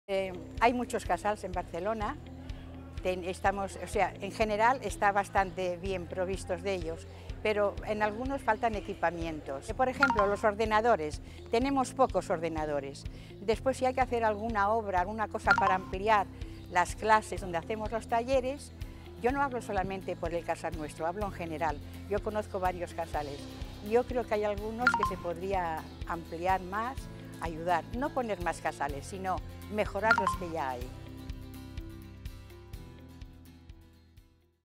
Aquí hi ha debat, aquí hi ha ciutat.
Entre tots els veïns i les veïnes volem construir col·lectivament una ciutat de drets i oportunitats. Què proposes per millorar els equipaments per a la gent gran. Entra a